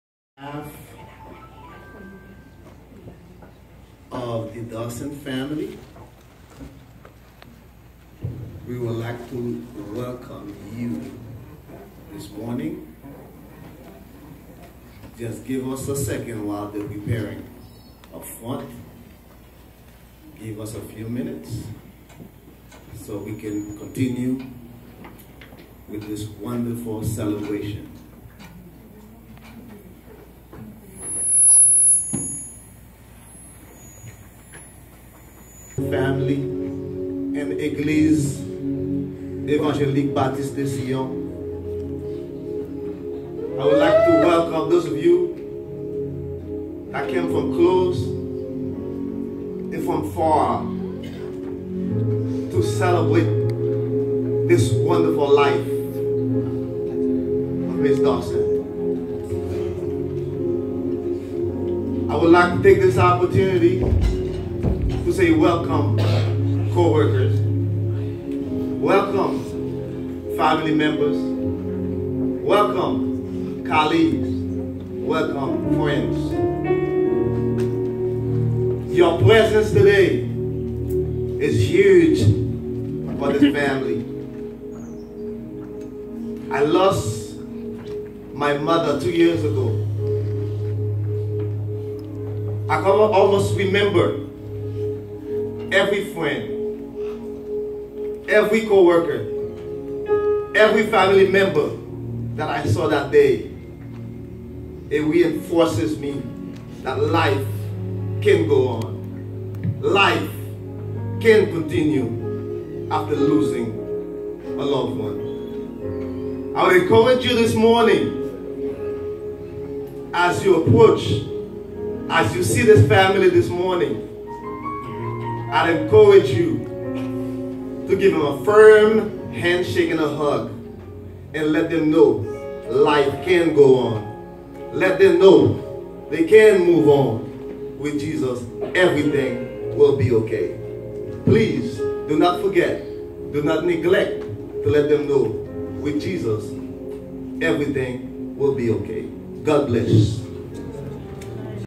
04 13 2024,CELEBRATING THE LIFE OF JOCELYNE PIERRE DORSAINT February 3, 1979 -March 20240413 101656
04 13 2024, CELEBRATING THE LIFE OF JOCELYNE PIERRE DORSAINT
February 3, 1979 -March 18, 2024
SERVICE
Saturday April 13, 2024
Viewing: 9:00 AM - Service 10:00 AM
Eglise Evangelique Baptiste de sion
1045 29th  Street Orlando Florida  32829